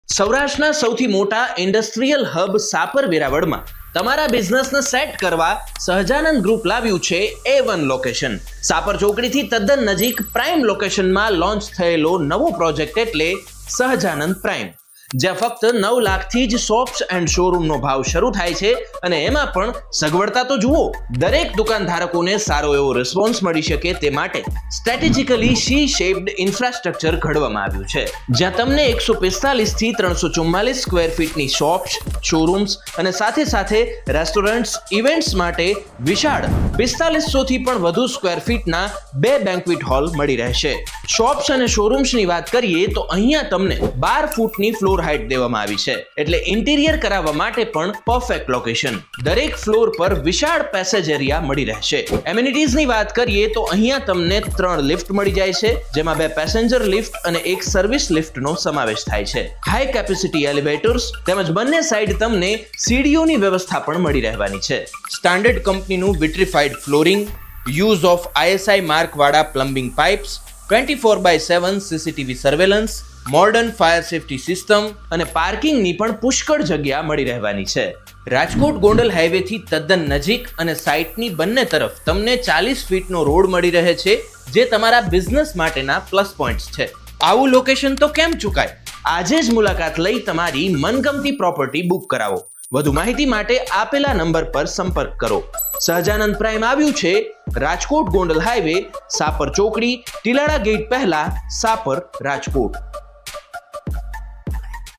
Sahjanand Prime 🏢 Shops•Showrooms•Banquet Halls
➡️ Sahjanand Prime 🏢

Shops•Showrooms•Banquet Halls

👉🏼Shop / Showroom Carpet - 144-344 Sq. Ft.
👉🏼Banquet Hall Carpet - 4500+ Sq. Ft.

⭕Vitrified Flooring
⭕ISI Marked Plumbing Pipes
⭕Spacious Parking
⭕Auto Door Lifts
⭕Modern Fire Safety System

📍Rajkot-Gondal Highway, Shapar Chowkdi, Before Tilada Gate, Shapar, Rajkot.